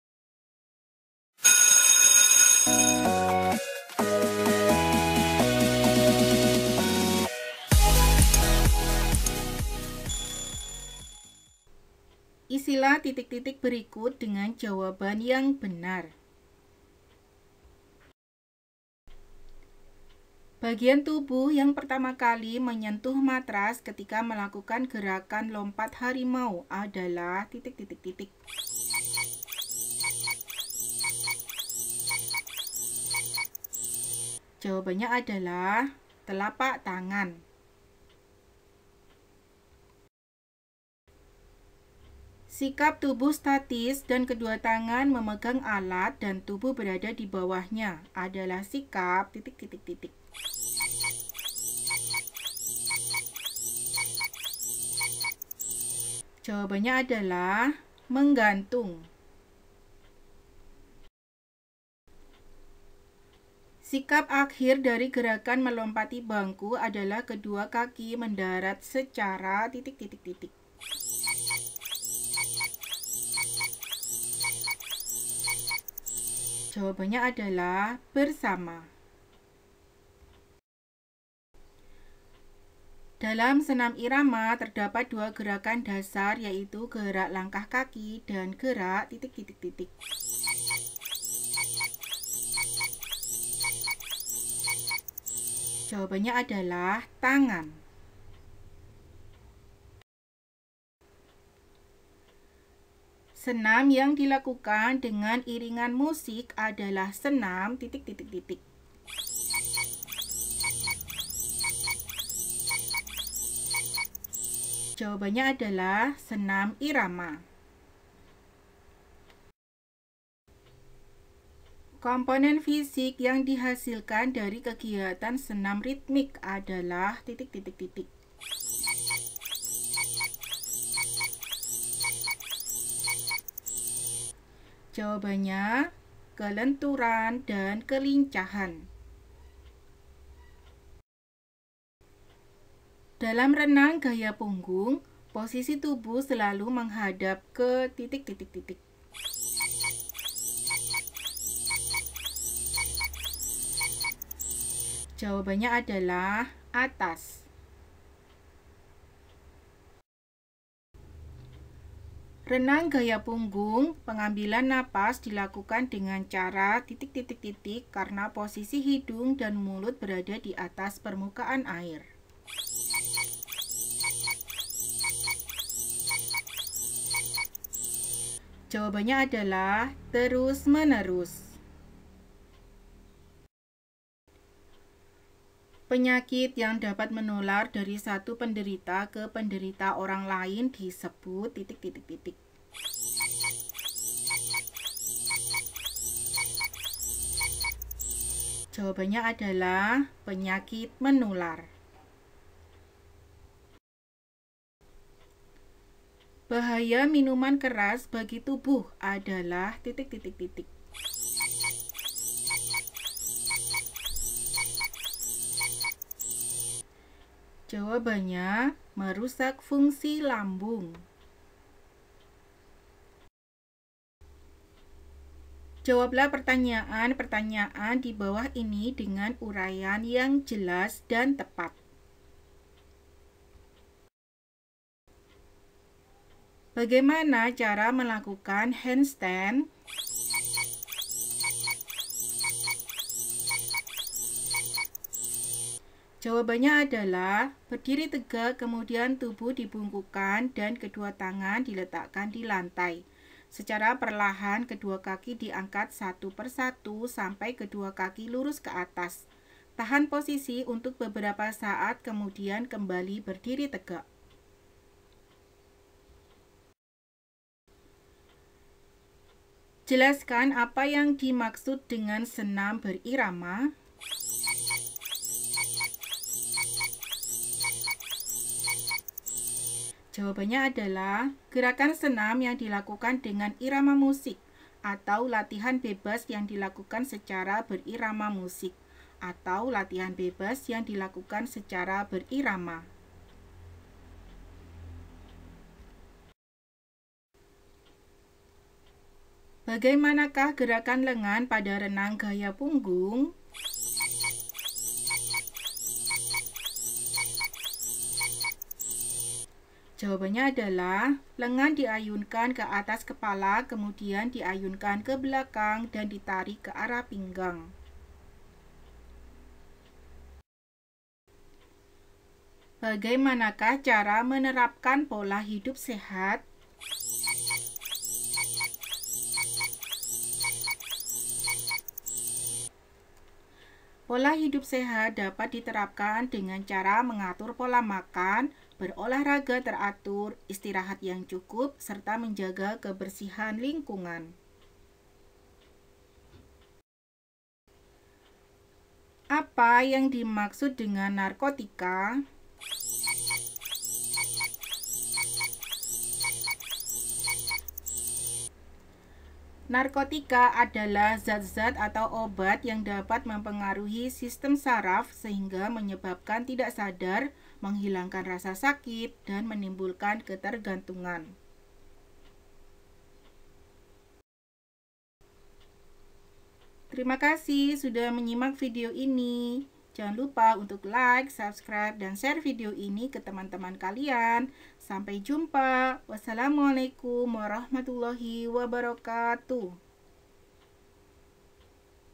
Soal PAT Kelas 5 SD PJOK (Bagian 2)
Video Pembahasan Latihan Soal PAT (Penilaian Akhir Tahun)  untuk mata pelajaran PJOK kelas 5 SD/MI

Semoga anak-anak lebih semangat untuk belajar :)
Terima kasih.

Playlist PAT Kelas 5 SD